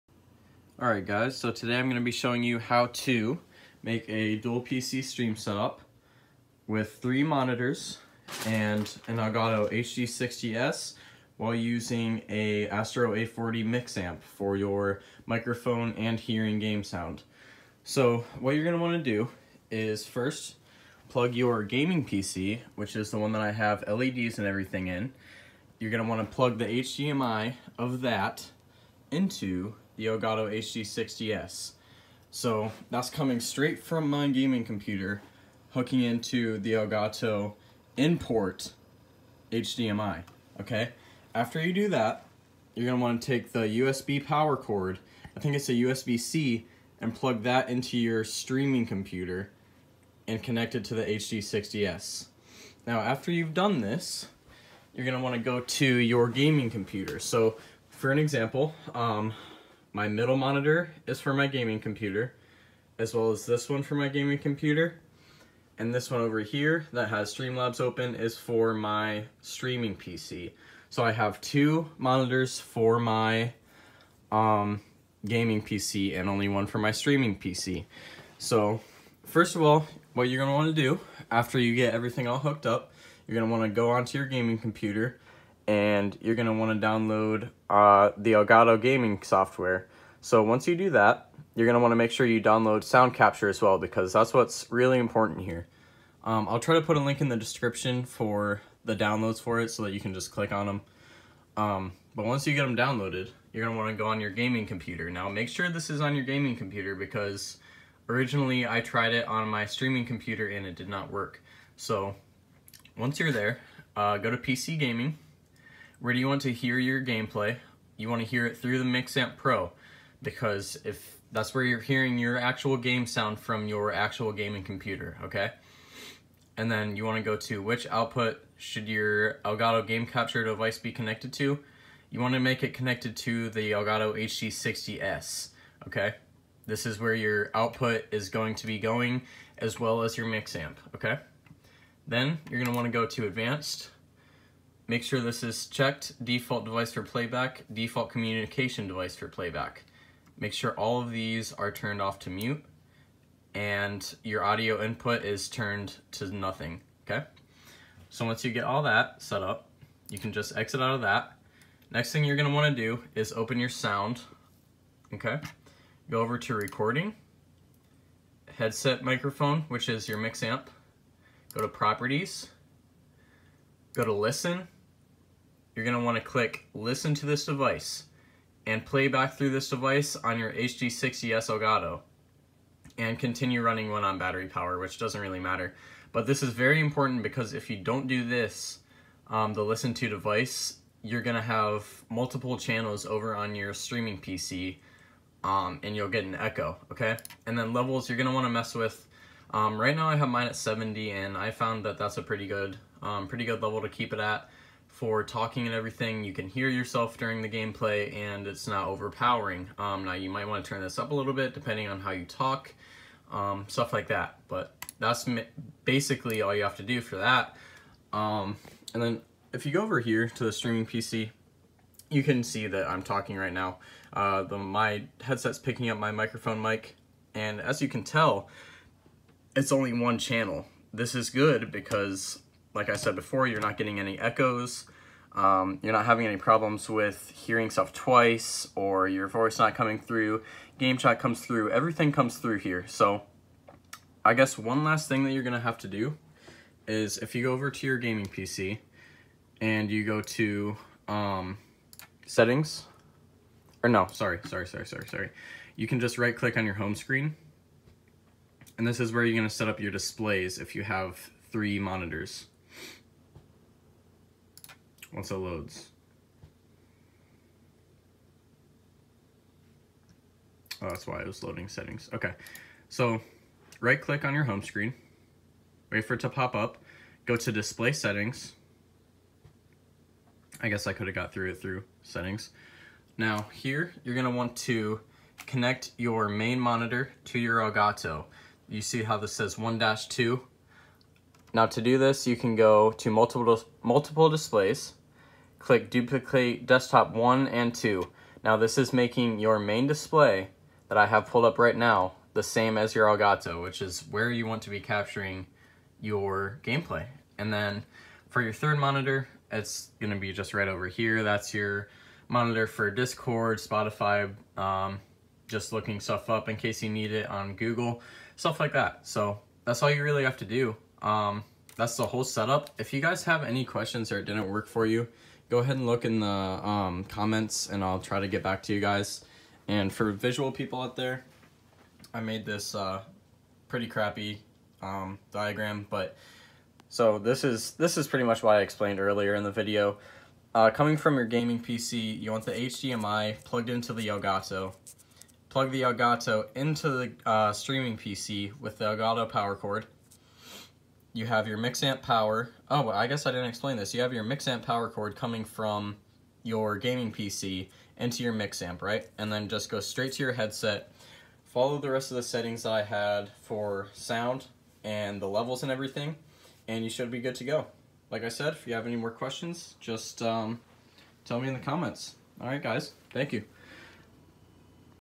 Dual PC Stream Setup Tutorial (w/ Elgato HD60 S & Astro Mixamp)
At the end of the video there is an explanation of how to set up the Astro mixamp/headset as well as a drawn out diagram for those of you who like to see that.

If you have any problems while setting this up, just comment on the video and I can try to help you out